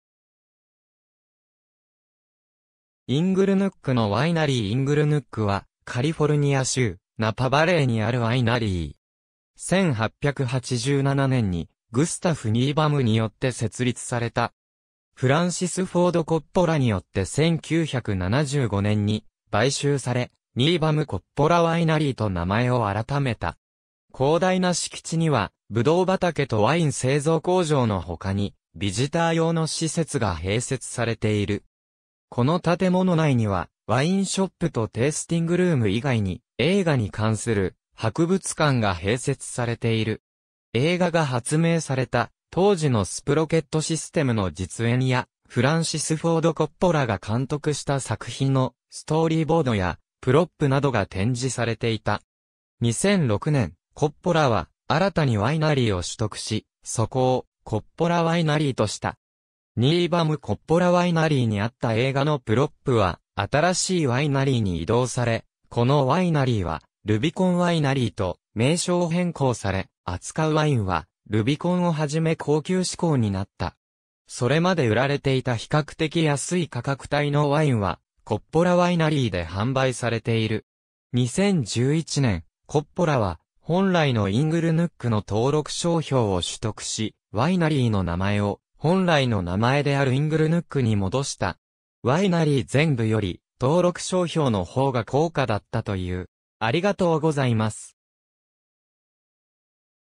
イングルヌック
イングルヌックのワイナリー イングルヌック (Inglenook) は、カリフォルニア州ナパ・バレーにあるワイナリー。
1887年にグスタフ・ニーバムによって設立された。
フランシス・フォード・コッポラによって1975年に買収され、「ニーバム・コッポラ・ワイナリー」(Niebaum-Coppola Winery)と名前を改めた。
広大な敷地には、葡萄畑とワイン製造工場のほかに、ビジター用の施設が併設されている。
この建物内には、ワインショップとテイスティングルーム以外に、映画に関する博物館が併設されている。
映画が発明された当時のスプロケット・システムの実演や、フランシス・フォード・コッポラが監督した作品のストーリーボードやプロップ（『タッカー』の車体、『ゴッドファーザー』の机、『ワン・フロム・ザ・ハート』の巨大なカクテルグラス）などが展示されていた。
2006年、コッポラは、新たにワイナリーを取得し、そこをコッポラ・ワイナリーとした。
ニーバム・コッポラ・ワイナリーにあった映画のプロップは新しいワイナリーに移動され、このワイナリーはルビコン・ワイナリーと名称変更され、扱うワインはルビコンをはじめ高級指向になった。
それまで売られていた比較的安い価格帯のワインは、コッポラ・ワイナリーで販売されている。
2011年、コッポラは本来のイングルヌックの登録商標を取得し、ワイナリーの名前を本来の名前であるイングルヌックに戻した。
ワイナリー全部より登録商標の方が高価だったという。